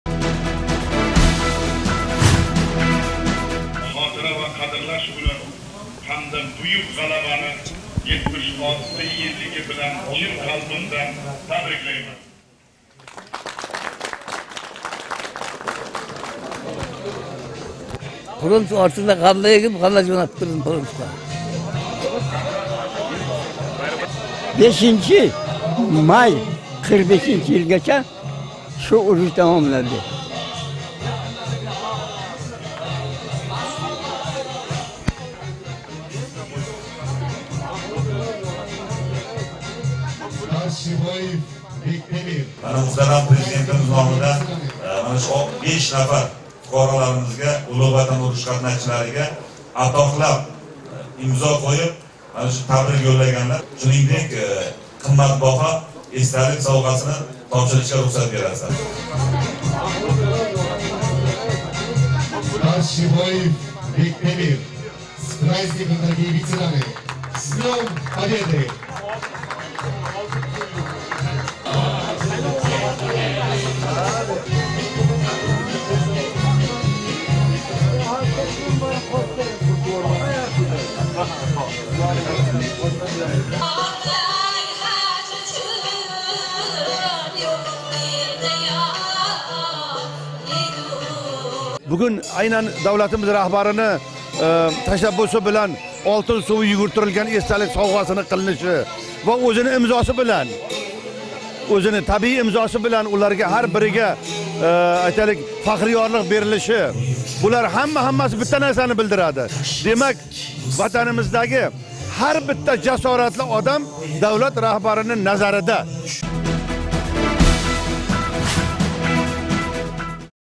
В Самарканде отметили 9 мая
В честь 9-мая-Дня памяти и почестей для ветеранов Второй мировой войны в ресторане «Багишамал Гарден» в Самарканде областной администрацией организовано праздничное мероприятие. В ходе торжества защитникам родины зачитали поздравительное письмо главы государства и вручили президентские подарки.